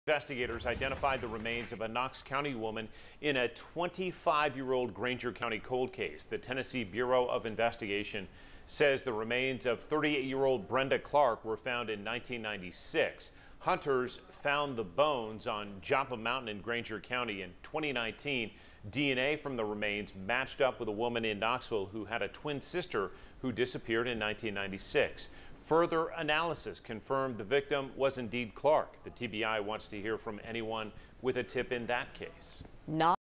DNA testing confirms bones found in 1996 belong to Knoxville homicide victim
Brenda Clark's remains were found in 1996 by hunters. The TBI is seeking information about who may have killed her.